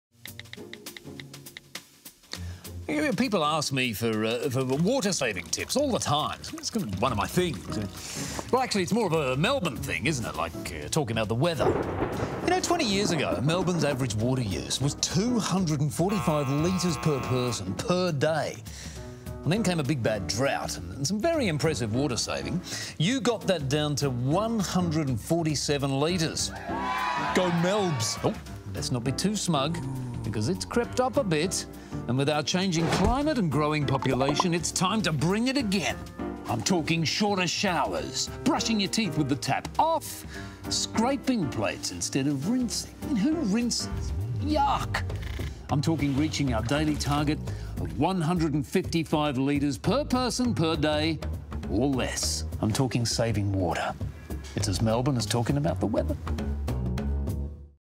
Water saving tips from Andrew Hansen
Melburnians proved we can be water wise during the Millennium Drought. Andrew Hansen explains how we can all do our bit and reach our target of 155 litres of water per person per day.